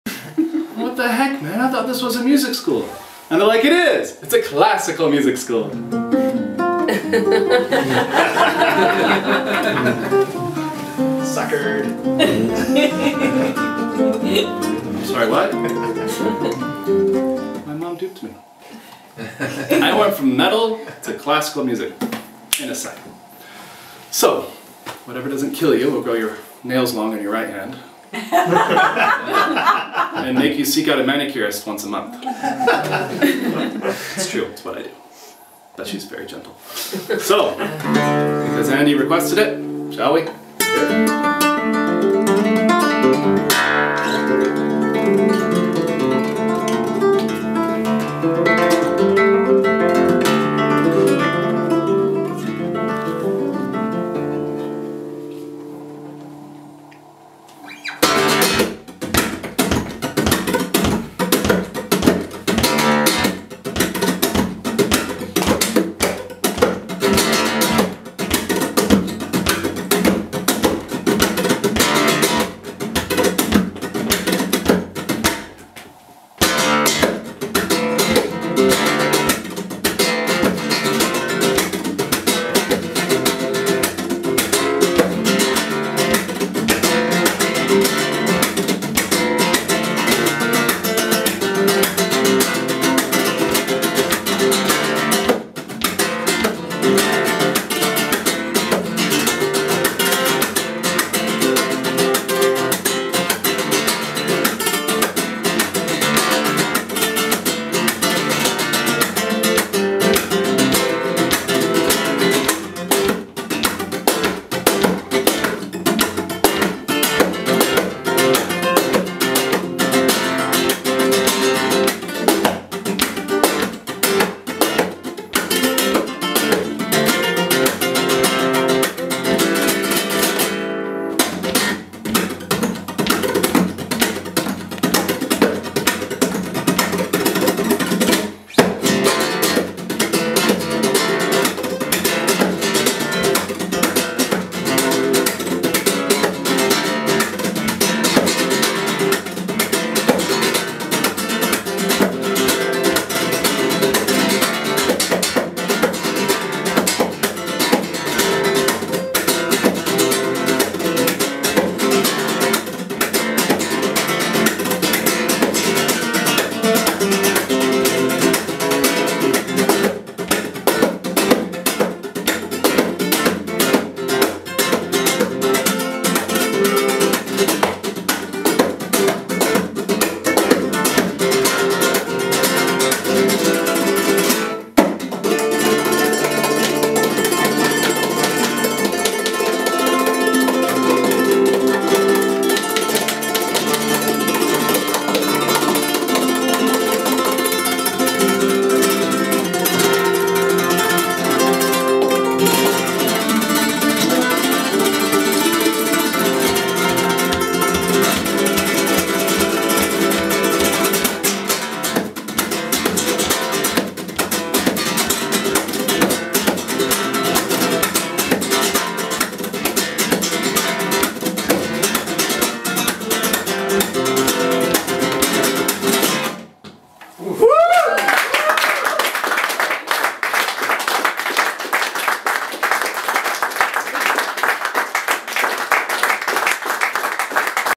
The Amazing Eric Harper with this amazing untitled track
Eric at at his July's Victoria house Concert B's perfomance wowing us all!